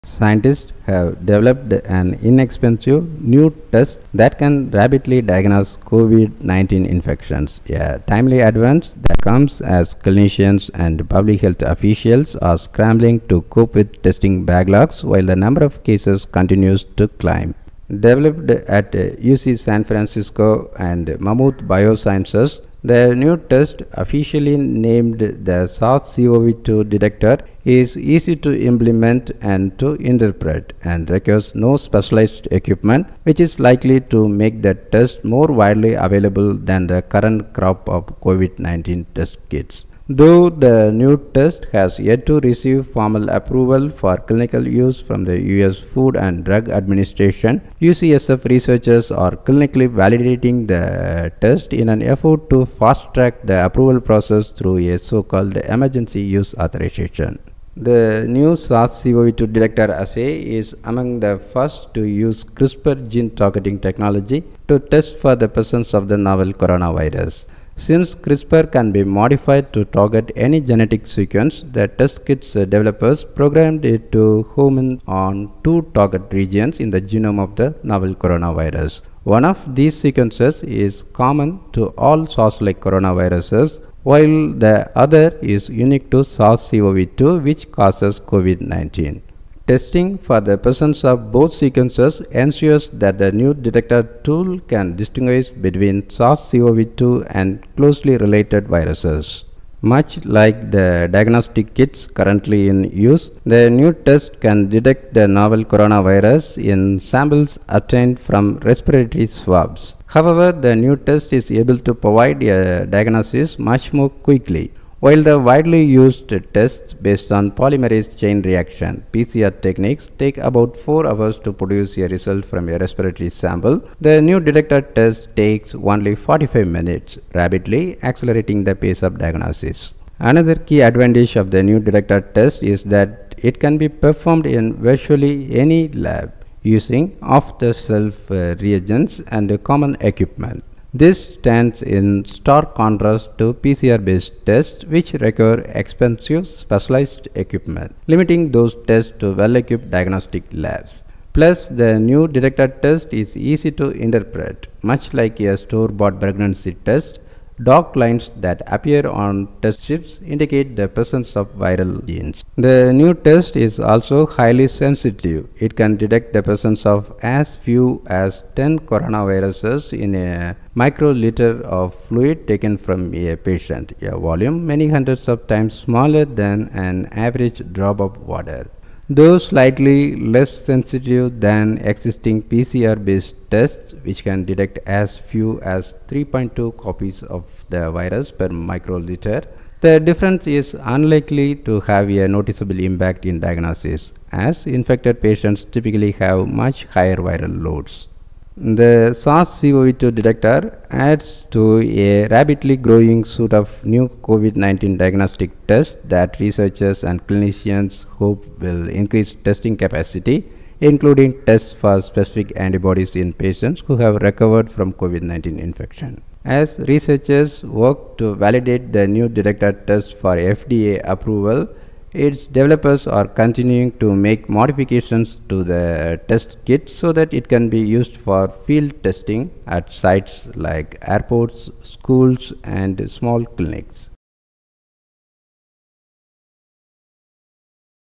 New CRISPR-Based COVID-19 Test Kit Can Diagnose Infection in Less Than an Hour
Scientists have developed an inexpensive new test that can rapidly diagnose COVID-19 infections, a timely advance that comes as clinicians and public health officials are scrambling to cope with testing backlogs while the number of cases continues to climb.

Developed at UC San Francisco and Mammoth Biosciences, the new test – officially named the “SARS-CoV-2 DETECTR” – is easy to implement and to interpret, and requires no specialized equipment, which is likely to make the test more widely available than the current crop of COVID-19 test kit.

Image credit: NIAID